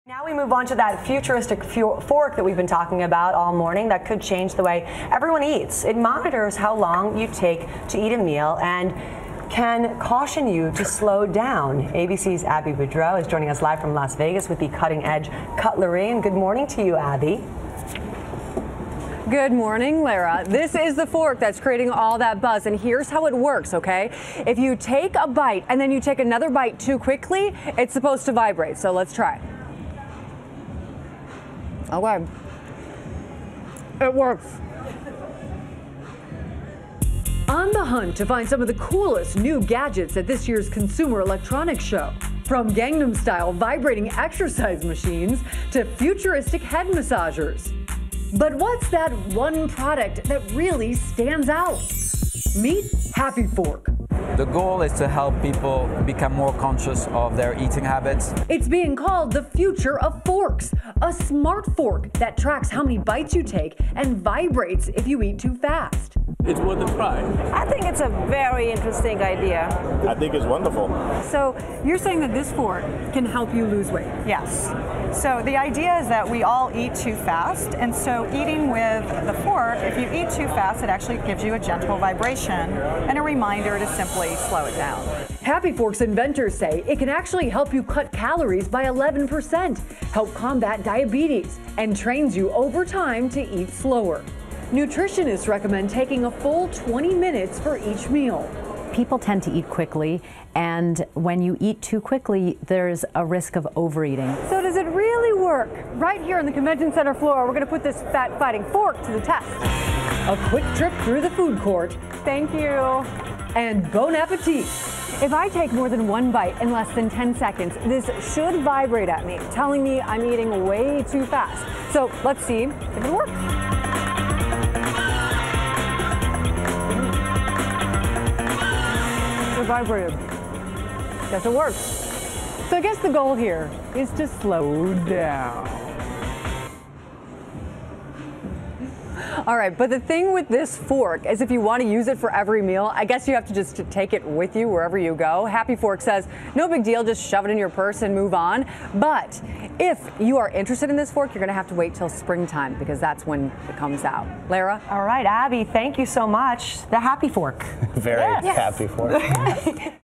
CES 2013: Vibrating Fork Monitors Eating Habits
A new fork unveiled at the 2013 Consumer Electronics Show can help cut calories by tracking how long it takes you to eat a meal.